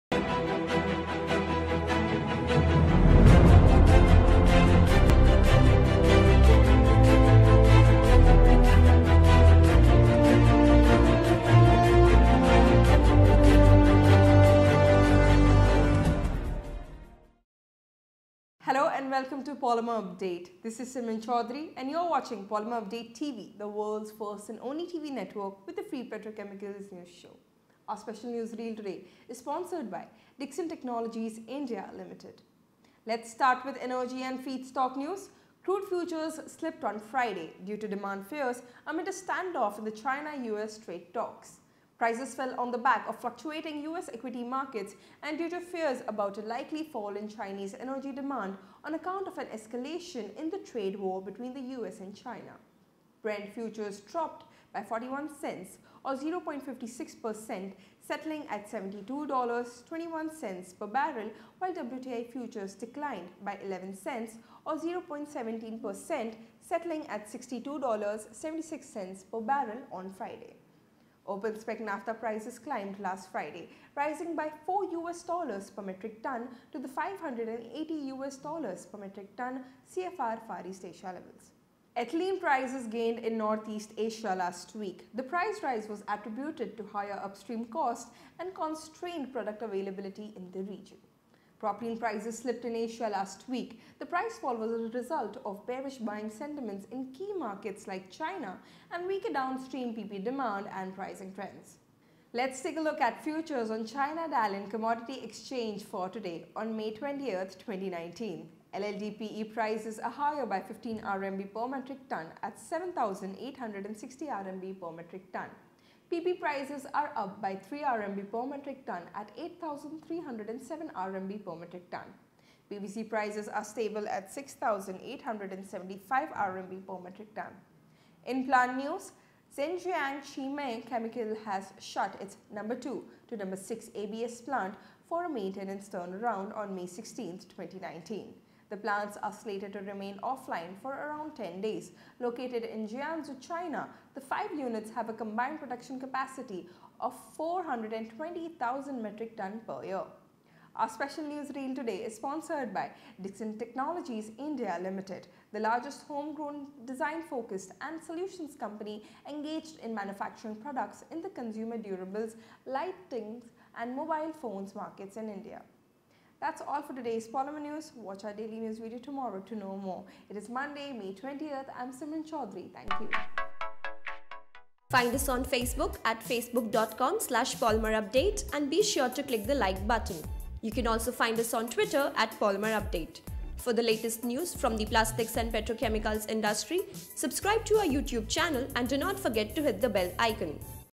Crude Oil Prices Edge Lower On Friday.
Today’s price changes in the global petrochemical industry with Simran Choudhary.